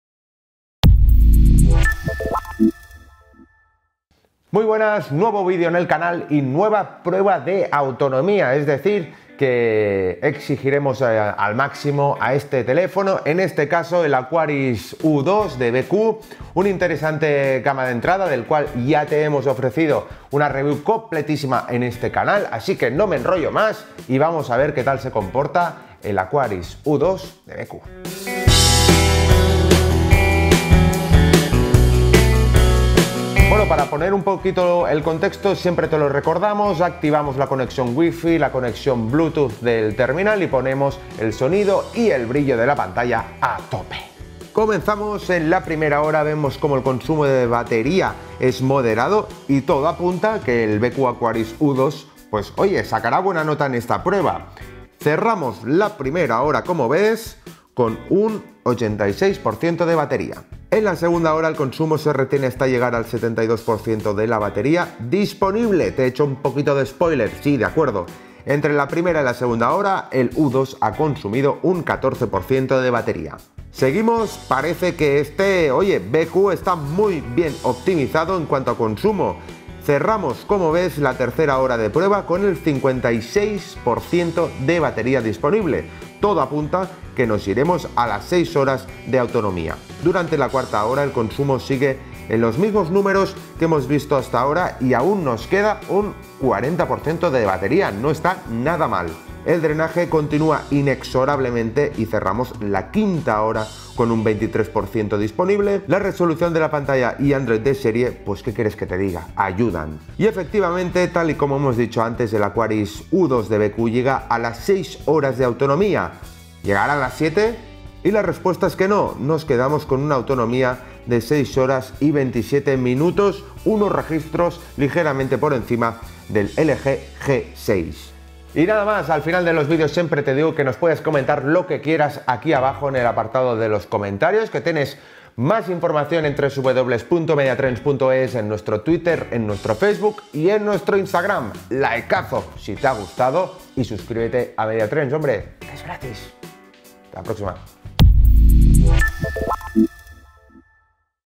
BQ AQUARIS U2. ¡Prueba de BATERIA!
¡Nueva prueba de batería en el canal! ¿Cuánto dura la batería del nuevo BQ Aquaris U2? No te pierdas nuestro video. :)